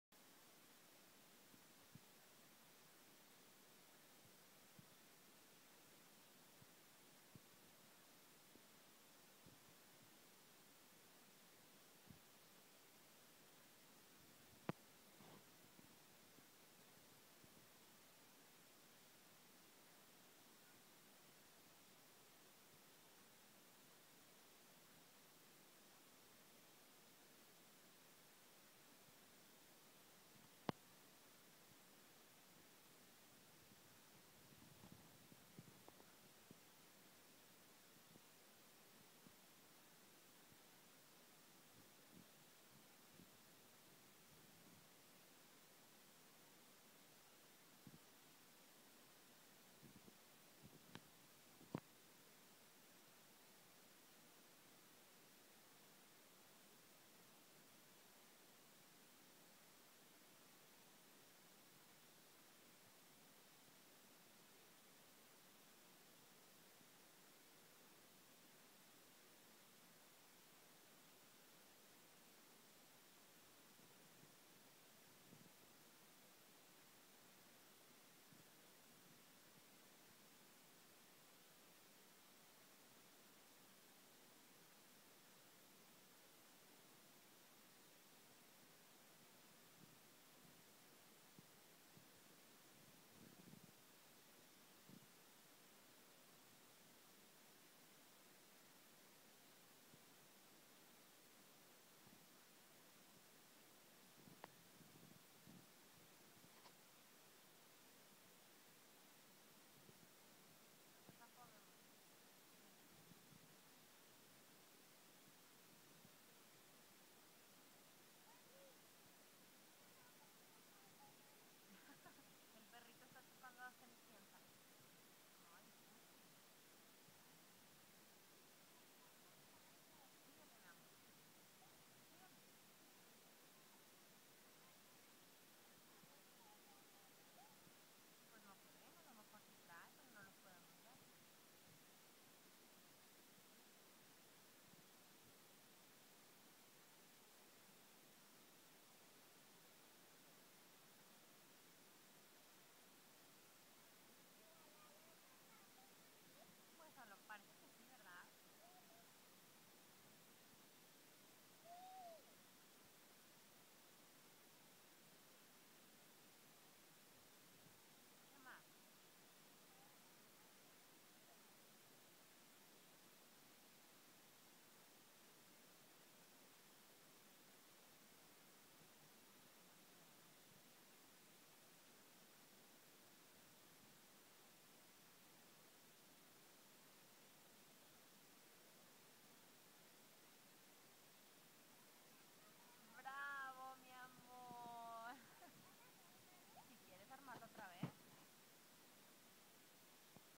Pmcs armando rompecabezas de 12 pzas
Cuando pau me dijo que quería un puzzle lo primero que pensé fue:"van a andar todas las piezas tiradas." me sorprendí cuando la vi solilla armandolo y ahora al tecer dia ya lo arma totalmente sola y mucho mas rápido. No hay que subestimar a los niños jamás!